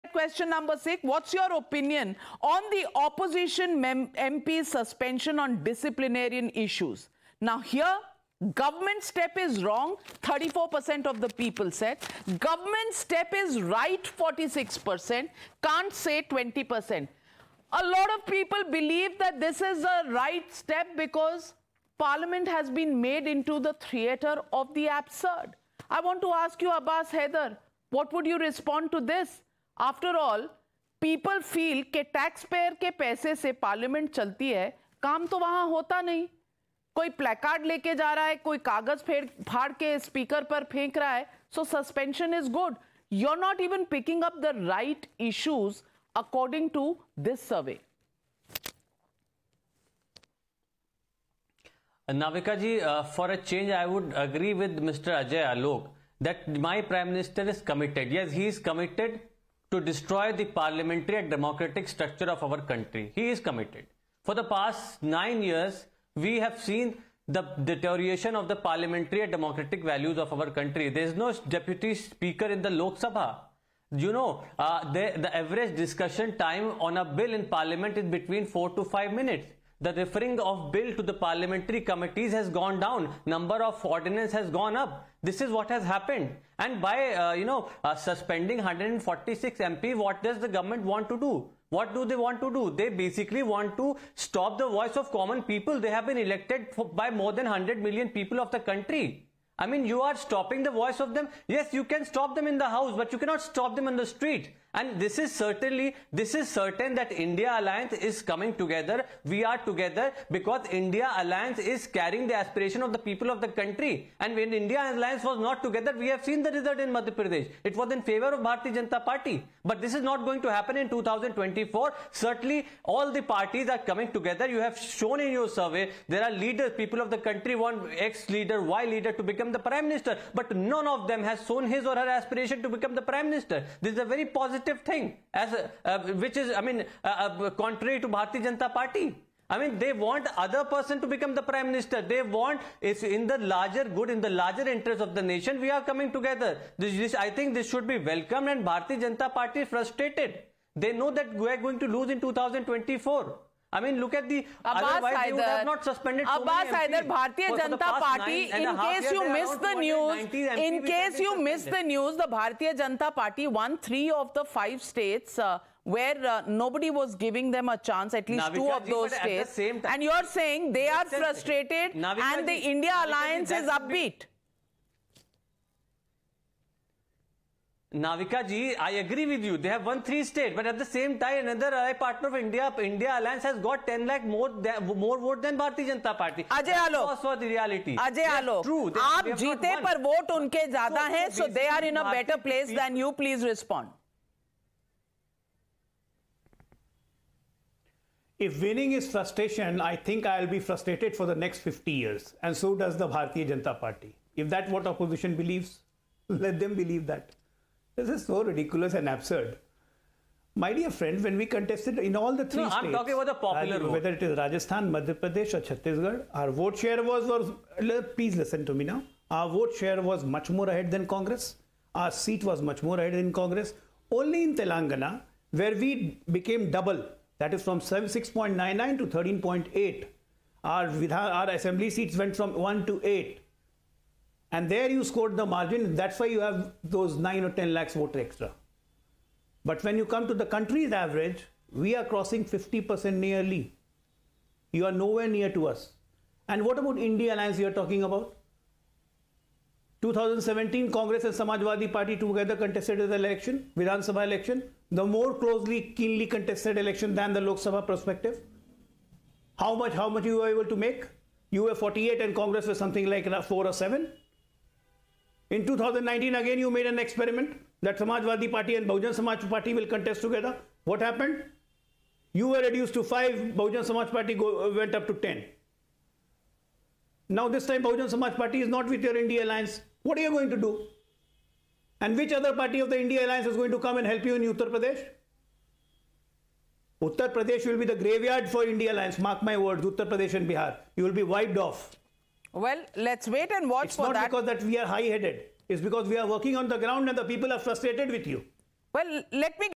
What's Your Opinion On Opposition MPs' Suspension On Disciplinarian Grounds? | Times Now ETG Survey
"I.N.D.I.A bloc is carrying the aspirations of this country," stated Abbas Haider of the Samajwadi Party.